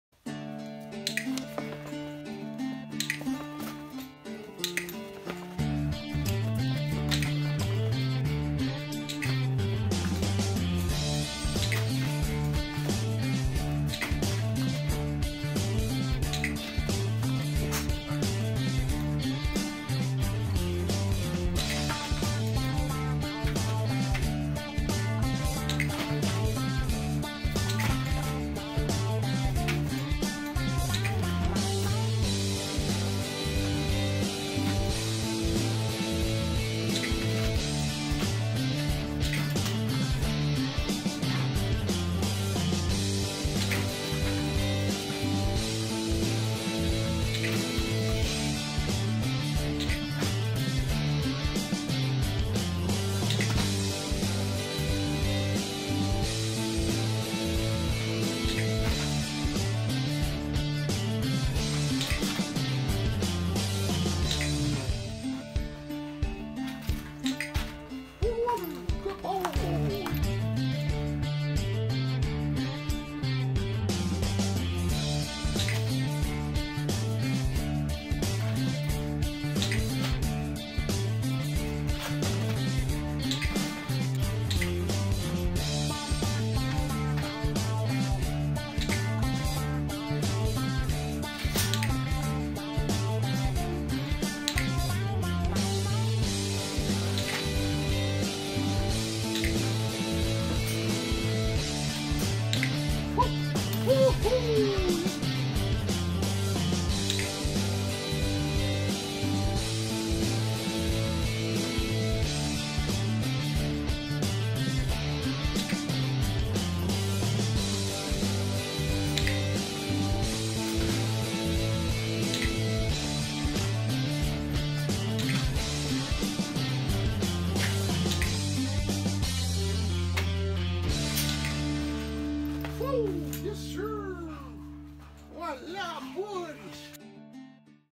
Huey's introduction to Odor
Today we introduced Huey to Black Powder as a target odor. One of the many reasons we like using a marker system is the high amount of repetitions we can get in. This video is a good example of that; this is one session and Huey’s first time on black powder. Huey is in our prospective police dog program and is available.